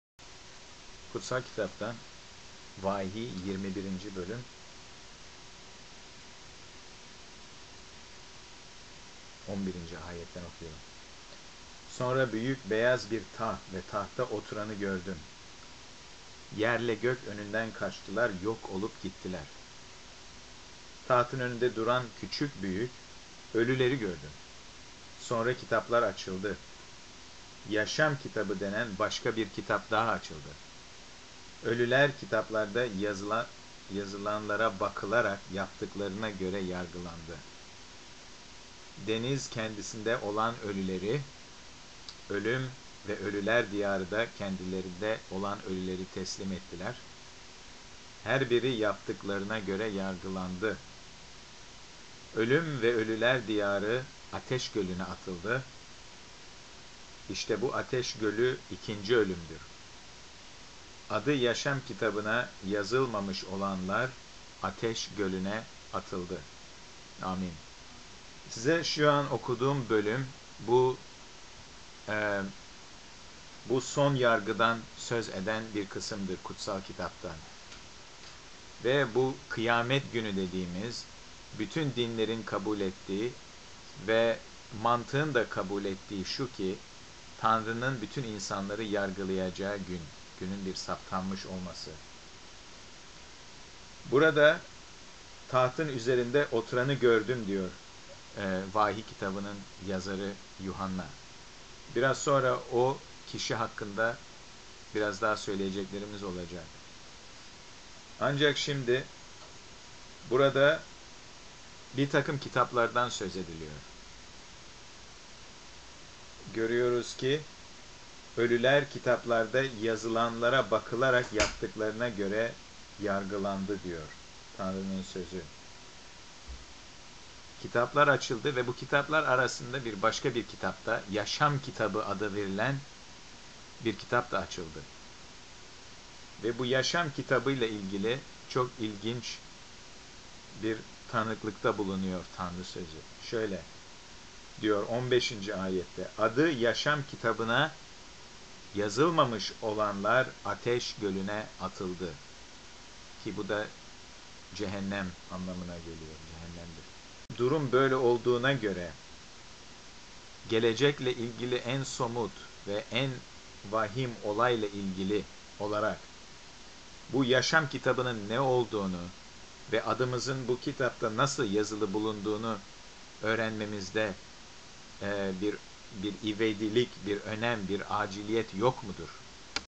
Kıyamet Günü 1inci Bölüm
(Vahiy20: 11) "Sonra büyük, beyaz bir taht ve tahtta oturanı gördüm. Yerle gök önünden kaçtılar, yok olup gittiler.[12] Tahtın önünde duran küçük büyük, ölüleri gördüm. Sonra kitaplar açıldı. Yaşam kitabı denen başka bir kitap daha açıldı. Ölüler kitaplarda yazılanlara bakılarak yaptıklarına göre yargılandı.[13] Deniz kendisinde olan ölüleri, ölüm ve ölüler diyarı da kendilerinde olan ölüleri teslim ettiler. Her biri yaptıklarına göre yargılandı.[14] Ölüm ve ölüler diyarı ateş gölüne atıldı. İşte bu ateş gölü ikinci ölümdür.[15] Adı yaşam kitabına yazılmamış olanlar ateş gölüne atıldı."